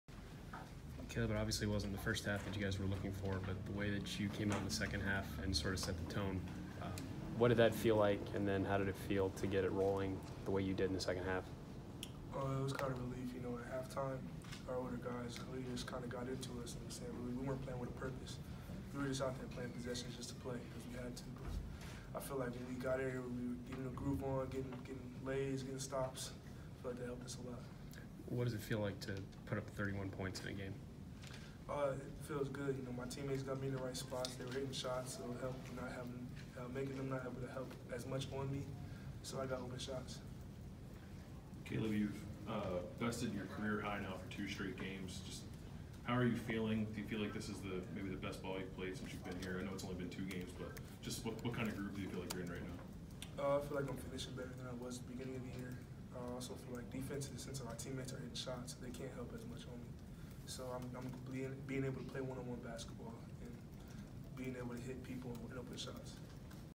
Ohio State sophomore Kaleb Wesson after scoring a career-high 31 points
Ohio State sophomore Kaleb Wesson after scoring a career-high 31 points in a 75-56 win against Youngstown State.